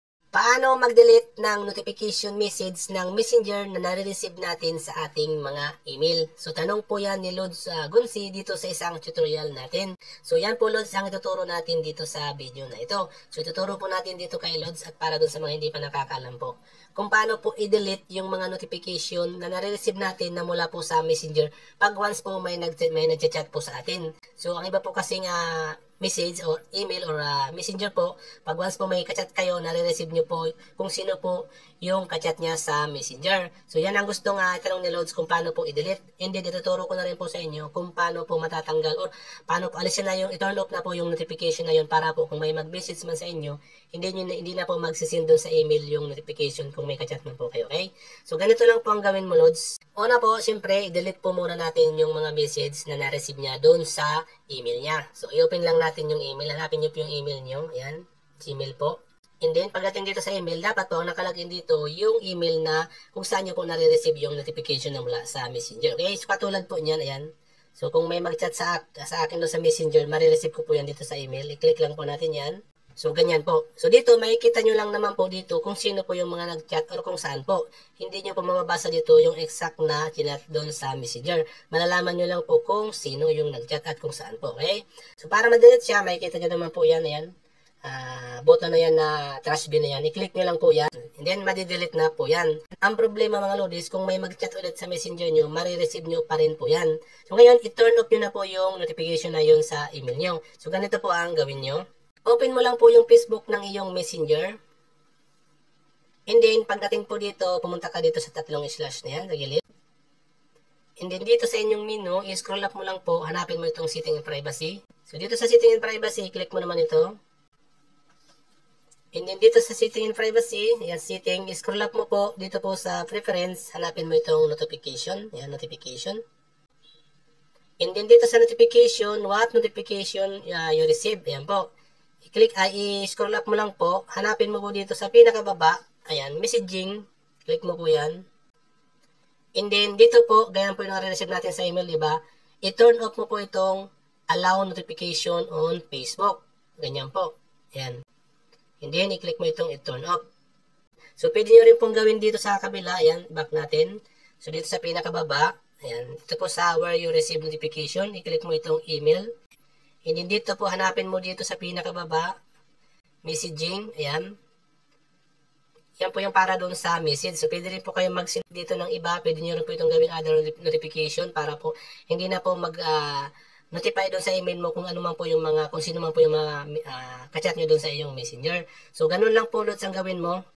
Paano mag DELETE ng Email na mula sa MESSENGER? | Turn Off Notification from Messenger to Email
Paano mag DELETE at TURN OFF ng NOTIFICATIONS MESSAGE sa EMAIL na mula sa MESSENGER?

Mga ka CHAT mo sa MESSENGER na re RECEIVED mo din ba sa EMAIL mo? Gusto mo TURN OFF para hindi na mag SEND dun ng NOTIFICATION? at Gusto mo din DELETE ang mga EMAIL na mula sa MESSENGER? Ito ang Tutorial para sayo lods.